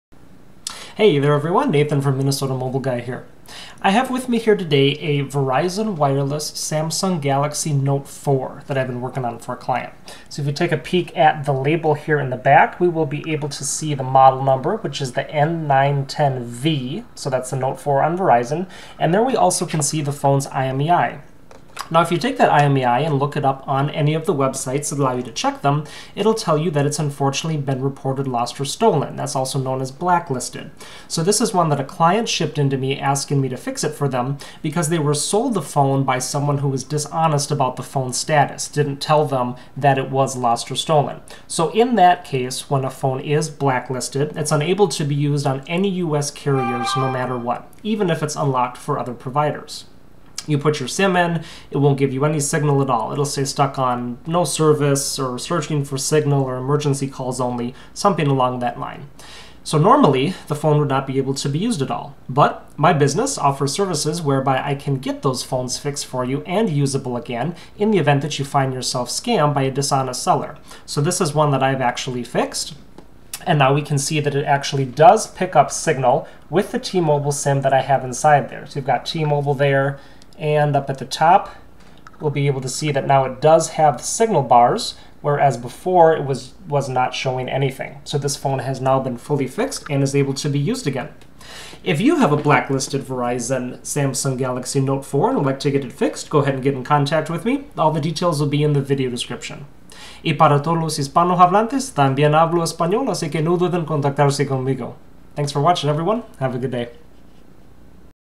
Blocked Blacklisted Verizon Wireless Samsung Galaxy Note 4 SM-N910V Fixed! (IMEI Repair)
Video showing a blacklisted Verizon Wireless Samsung Galaxy Note 4 (SM-N910V) which has been repaired to be usable again. If a phone is reported lost or stolen or if it's blocked for nonpayment, it won't have service when you pop a SIM in - it'll stay on "No service" or "Searching" or "Emergency calls only".